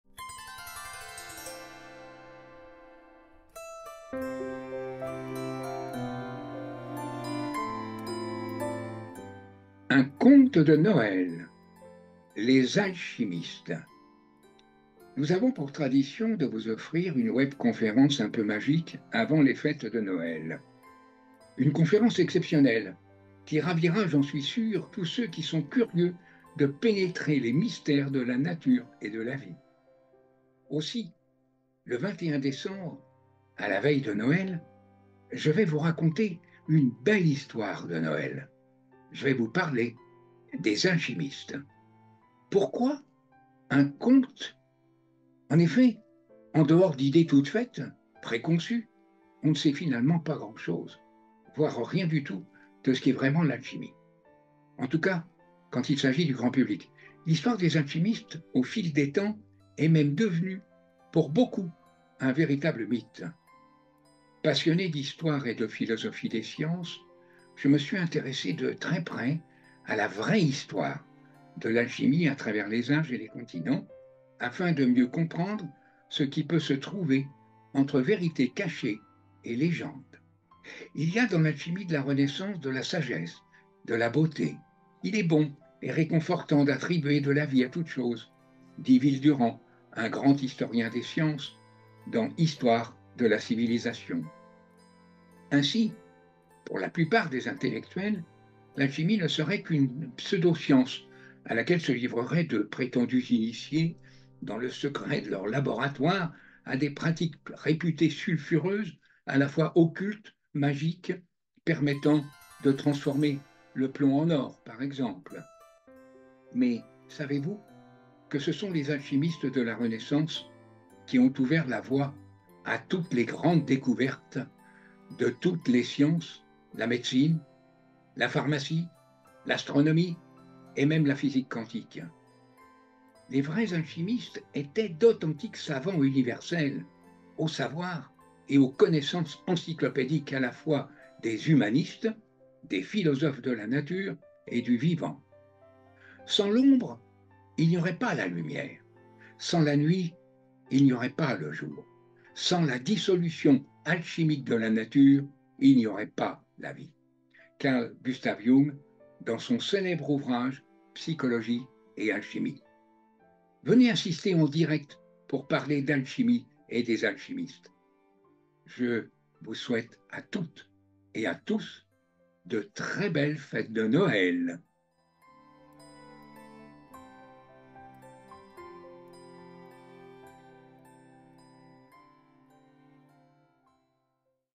Web conférence gratuite " UN CONTE DE NOËL : LES ALCHIMISTES ".
Nous avons pour tradition de vous offrir une web conférence un peu magique avant les fêtes de Noël.
Une conférence exceptionnelle qui ravira j’en suis sûr tous ceux qui sont curieux de pénétrer les mystères de la nature et de la vie.
Aussi le 21 décembre, à la veille de Noël, je vais vous raconter une belle histoire de Noel.
Je vais vous parler des alchimistes...